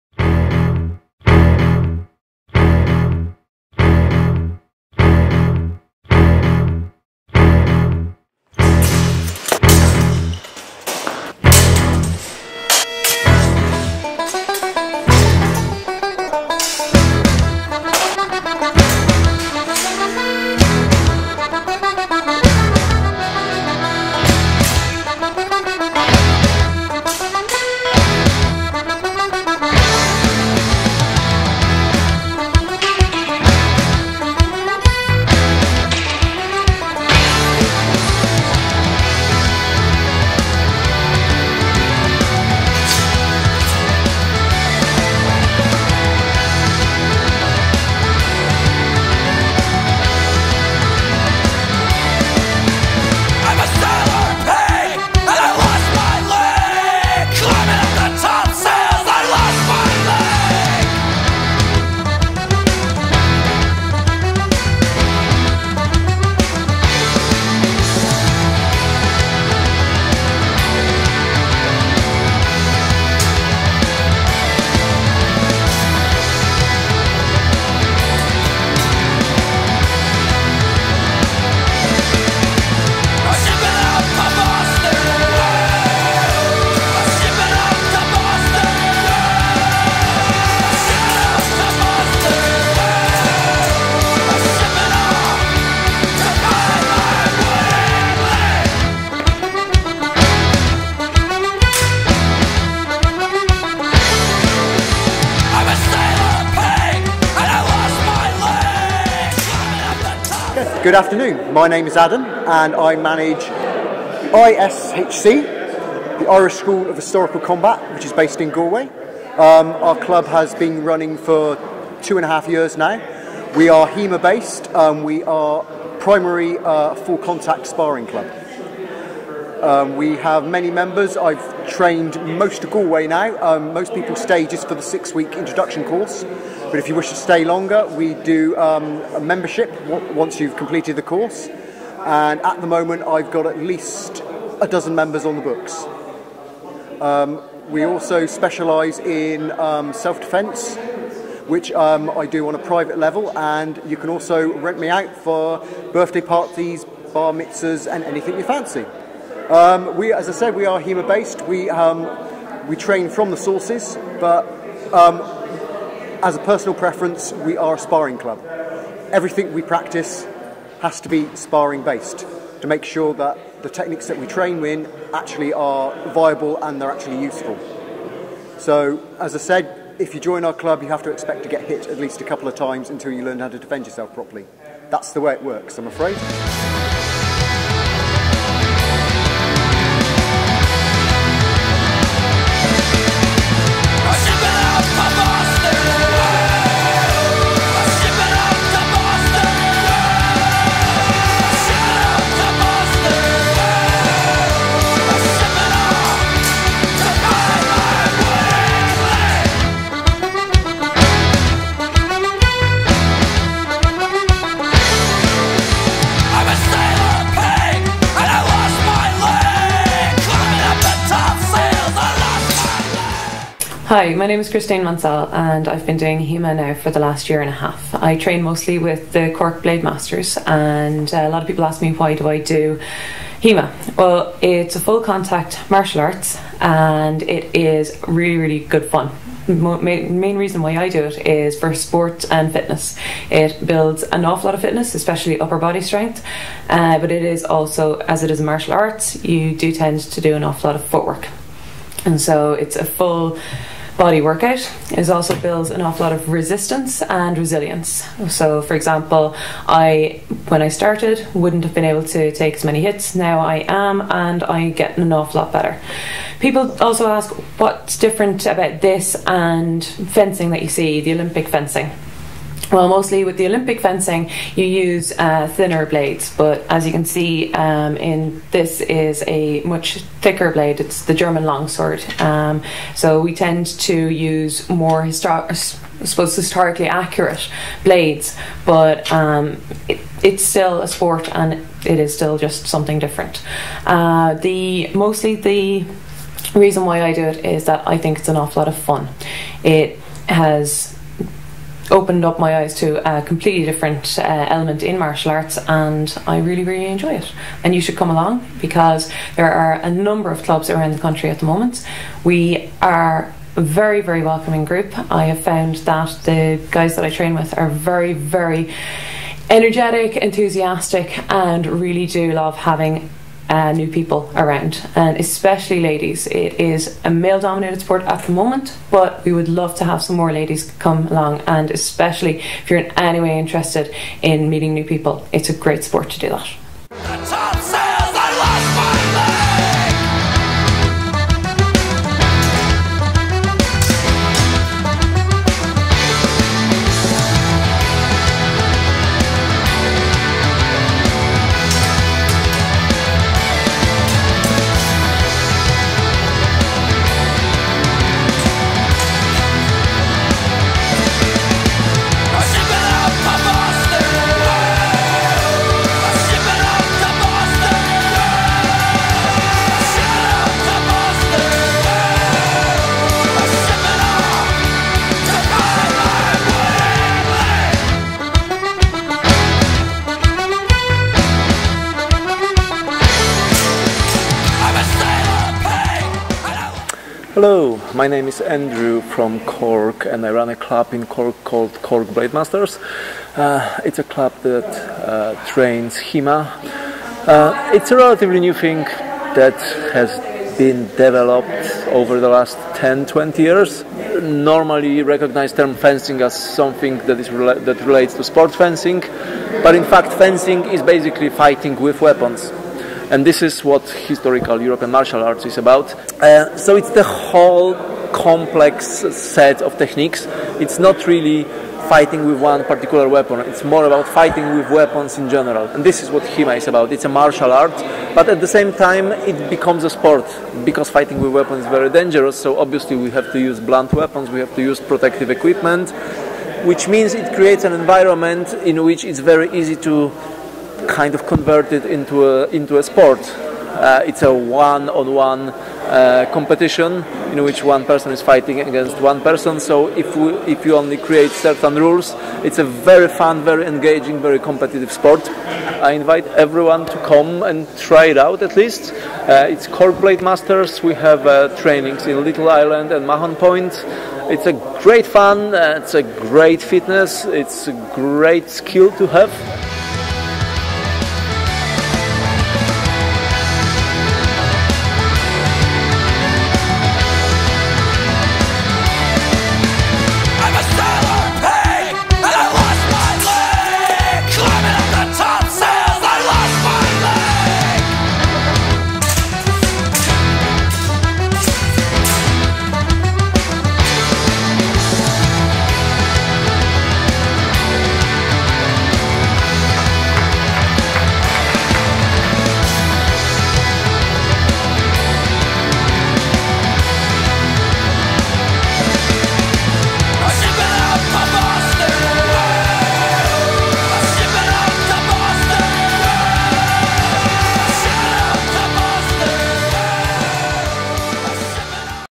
Hema Tournament. (Documentary)
Historical European Martial Arts. Popular sword fighting sport especially in eastern europe but now spreading to other countries.